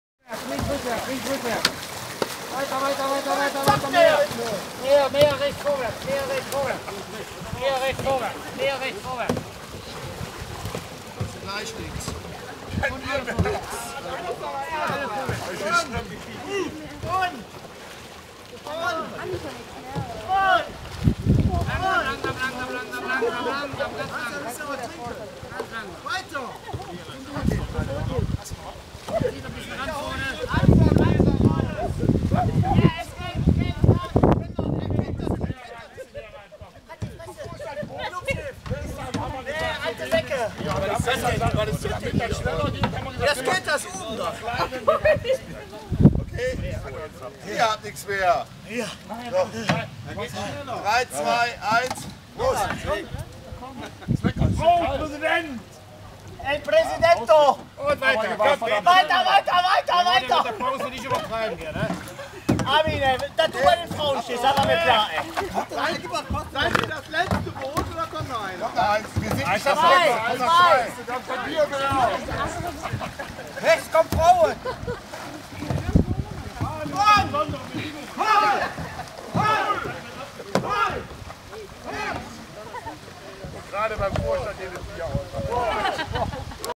Wevelinghoven Canadierrennen 2015 - "Alte Säcke" wendet
Wendemanöver der "Alten Säcke" inclusibe "Bierstrafe" beim Canadierrennen anläßlich des Stadtparkfestes in Wevelinghoven.